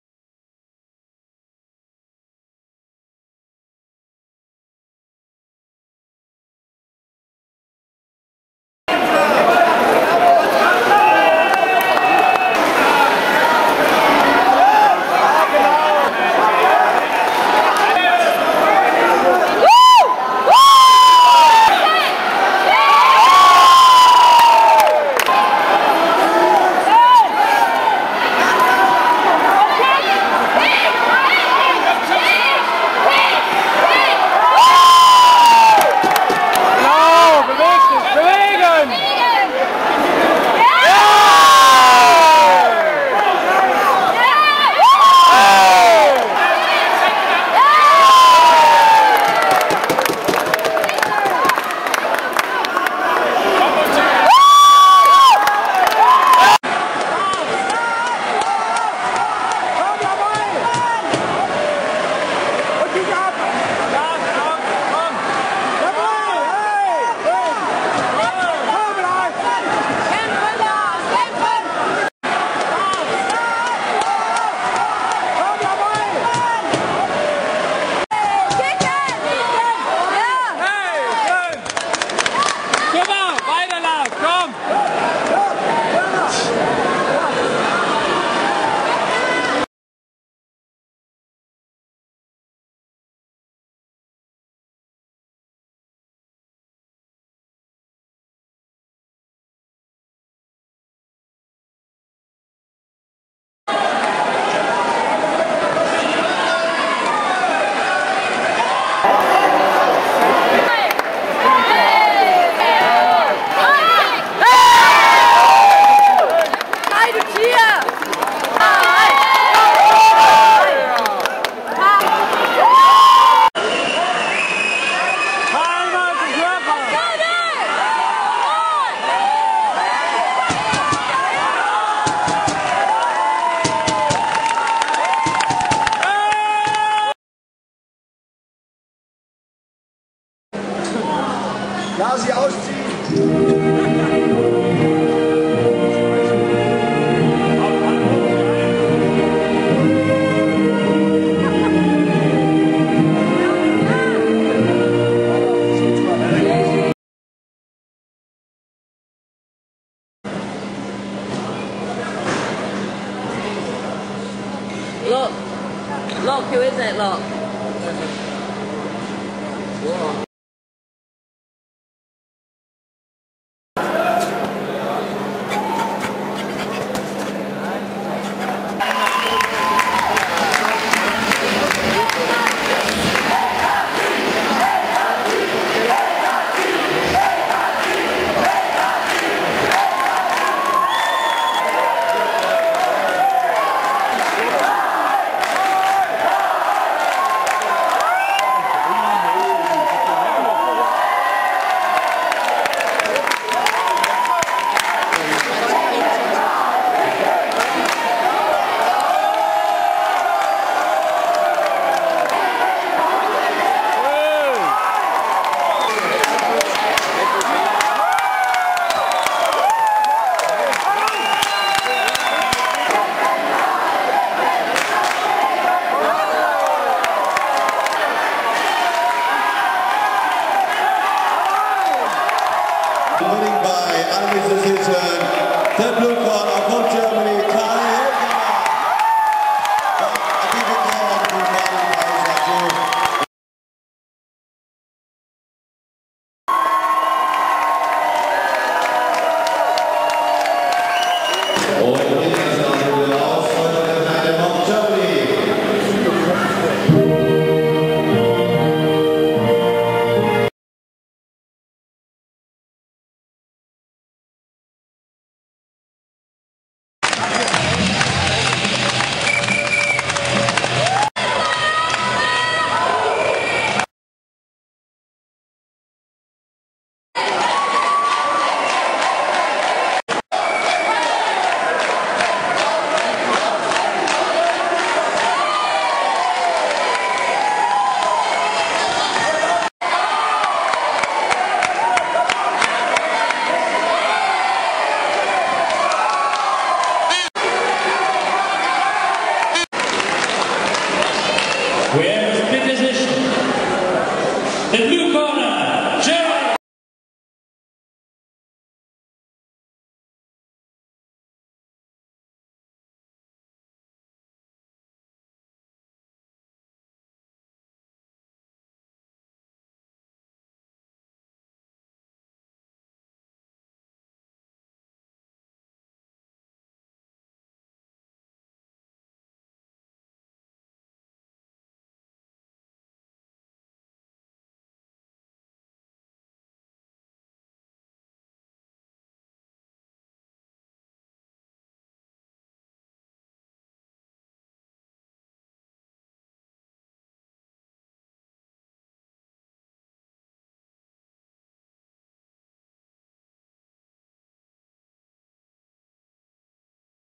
Trailer_lang_WKA_WM_07_Kai Höfer_Weltmeister
WKA Kickbox WM 2007. Trailer der Kämpfe von Weltmeister Kai Höfer und seinem Zwillingsbruder Lars Höfer. Auch ein Kampfausschnitt von Thorsten Kornatzki ist zu sehen.